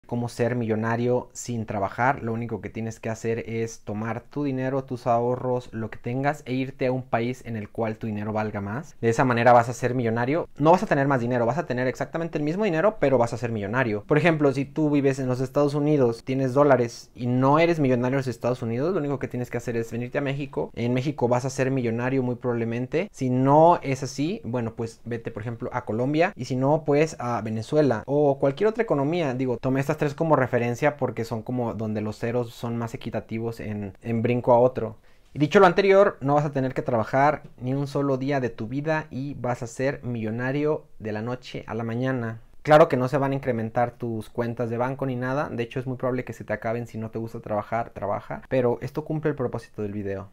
¿se puede ser millonario? | ¿sin trabajar?
0:00 Intro
0:11 ¿se puede ser millonario?
0:22 ¿sin trabajar?

como ser millonario sin trabajar lo único que tienes que hacer es tomar tu dinero tus ahorros lo que tengas e irte a un país en el cual tu dinero valga más de esa manera vas a ser millonario no vas a tener más dinero vas a tener exactamente el mismo dinero pero vas a ser millonario por ejemplo si tú vives en los estados unidos tienes dólares y no eres millonario si eeuu lo único que tienes que hacer es irte a méxico en méxico vas a ser millonario muy probablemente si no es así bueno pues vete por ejemplo a colombia y si no puedes a venezuela o cualquier otra economía digo tomé estas tres como referencia porque son como donde los ceros son más equitativos en un brinco a otro y dicho lo anterior no vas a tener que trabajar ni un solo día de tu vida y vas a ser millonario de la noche a la mañana claro que no se van a incrementar tus cuentas de banco ni nada de hecho es muy probable que se te acaben si no te gusta trabajar trabaja pero esto cumple el propósito del vídeo